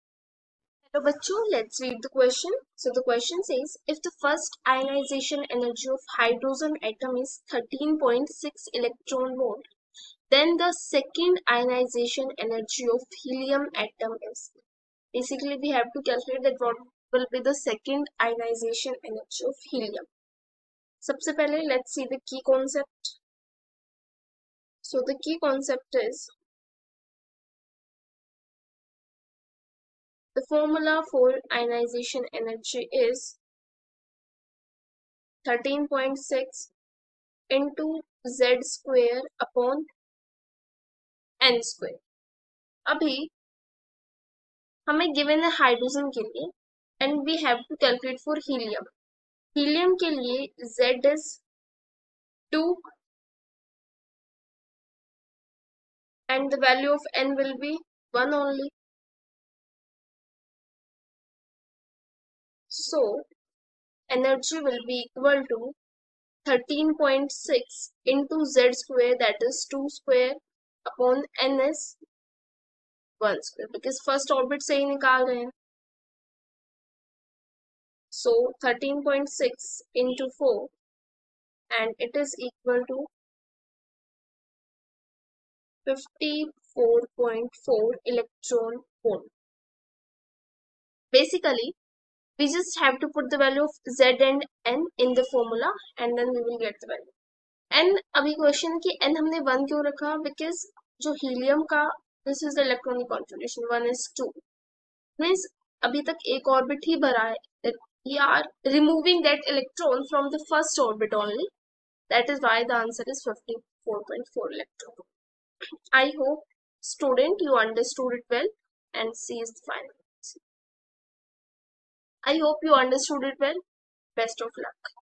If the first ionisation energy of H- atom is 13.6 eV, then the second ionisation energy of He- a....
If the first ionisation energy of H- atom is 13.6 eV, then the second ionisation energy of He- atom is: 📲PW App